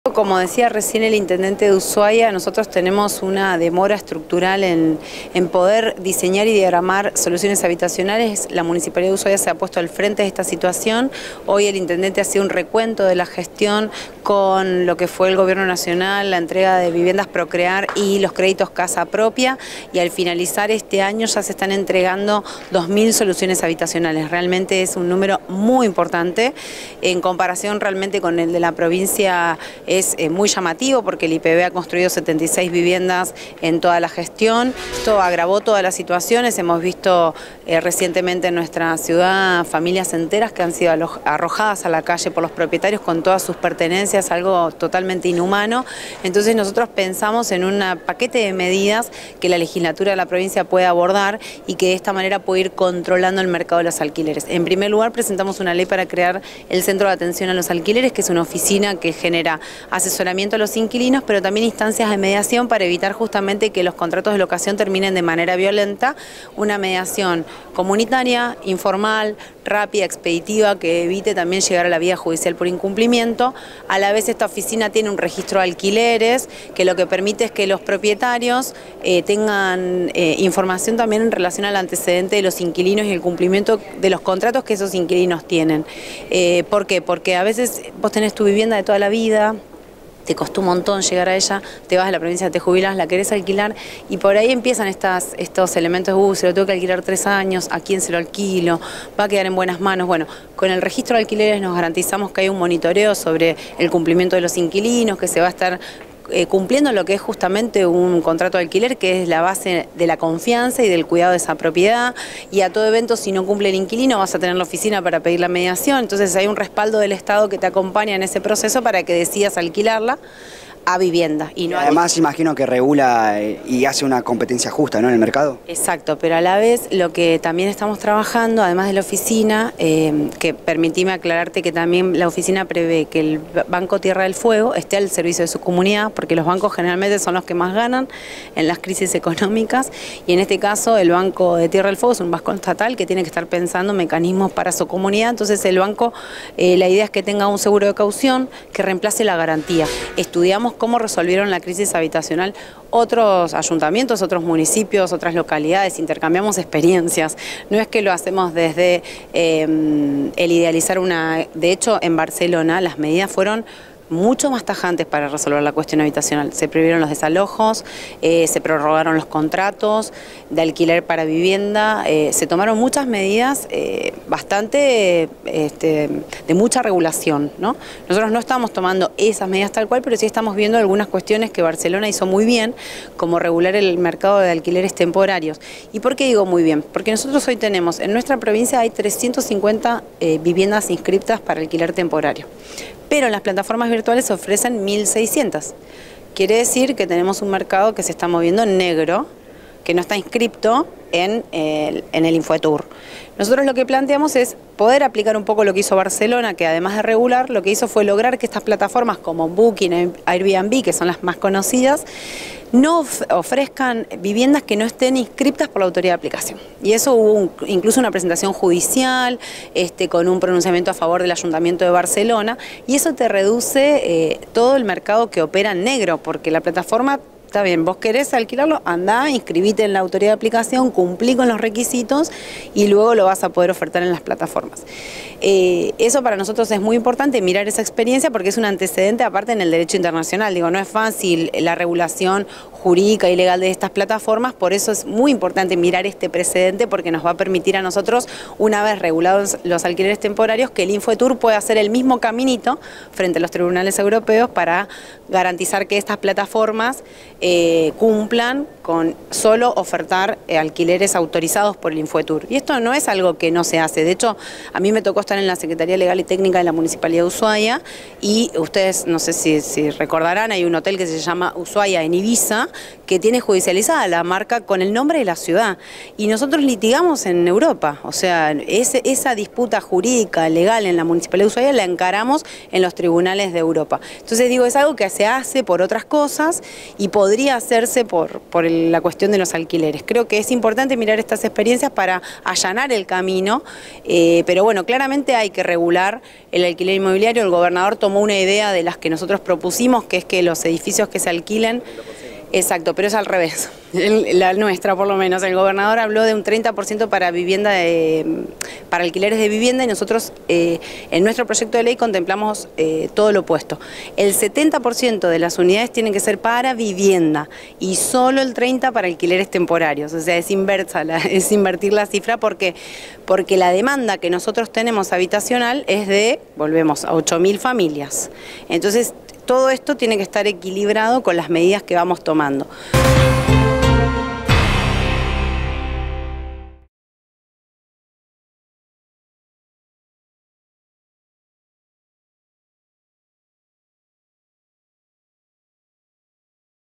“HAY UNA DEMORA ESTRUCTURAL”.
La legisladora Vuoto explicó su proyecto para tratar la demanda habitacional en la provincia.